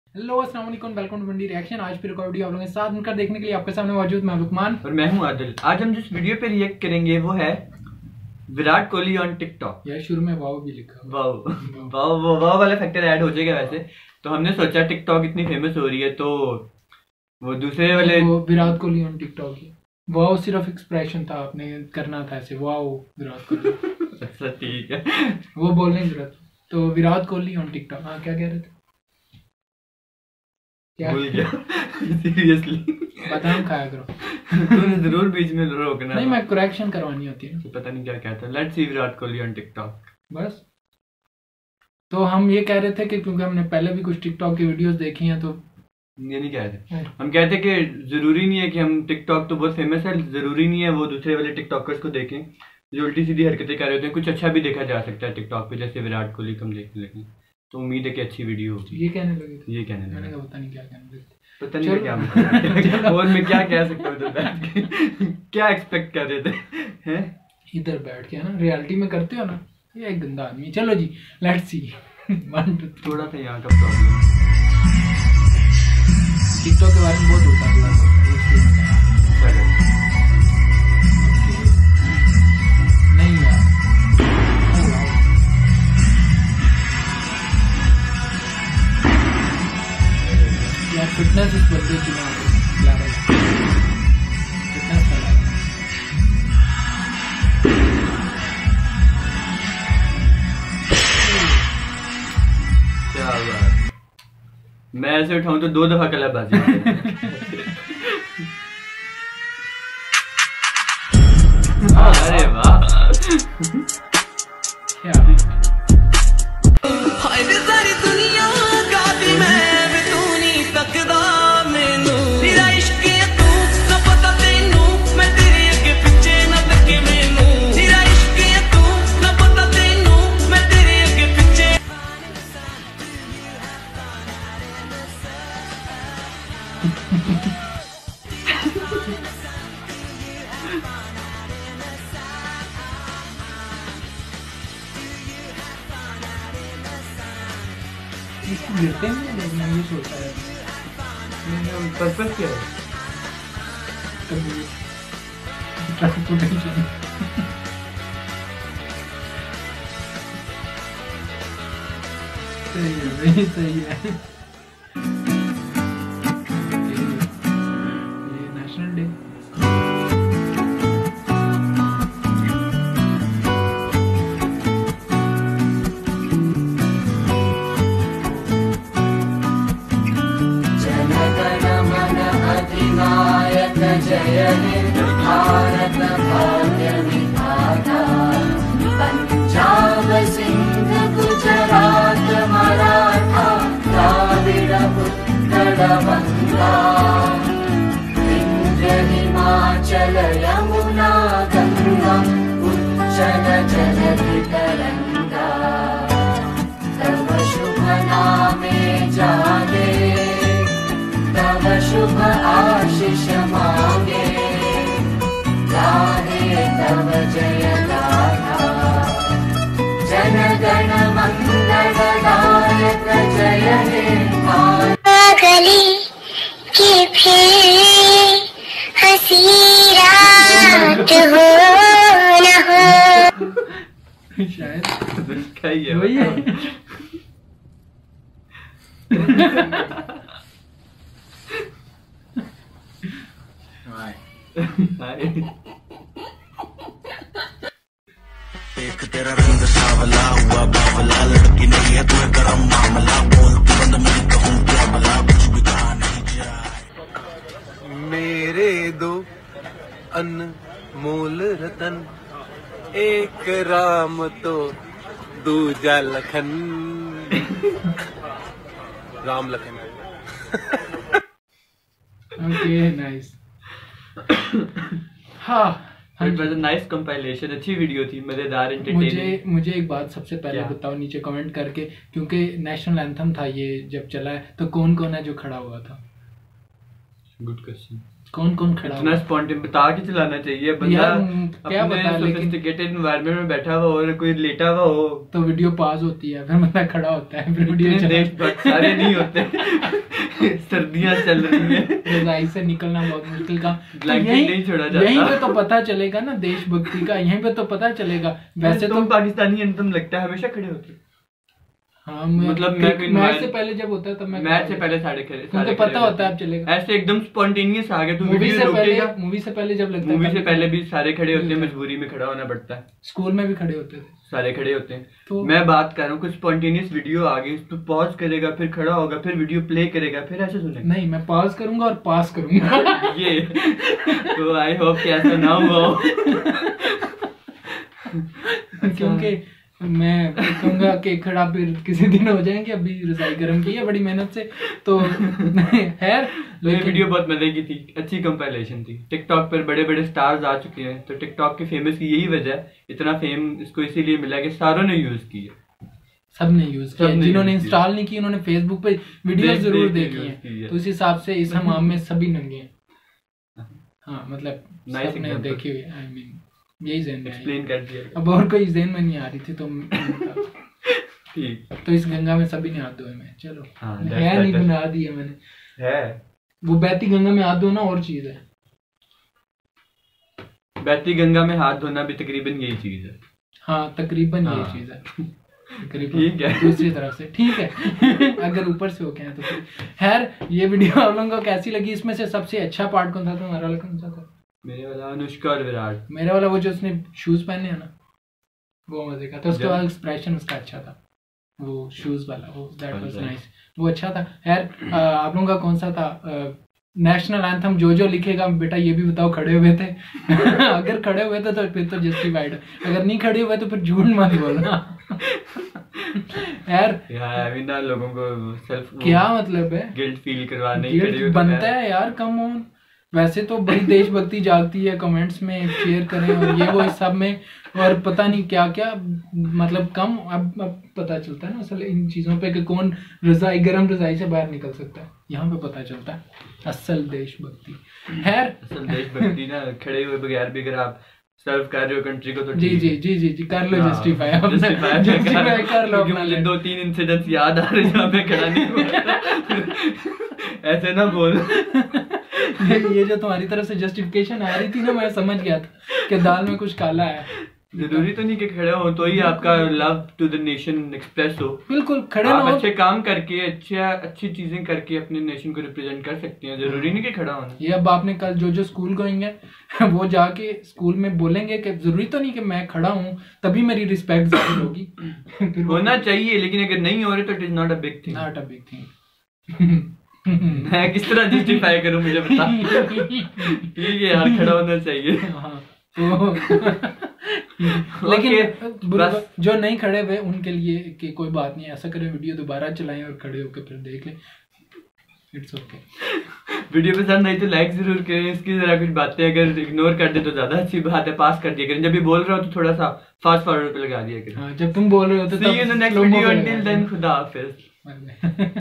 Pakistani Reaction To | WOW..VIRAT KOHLI ON TIKTOK
This is a Reaction Video of WOW..VIRAT KOHLI ON TIKTOK
Please do like it and share your views in Comments Section

Suggest the videos that you want us to react, and we will watch it with you here.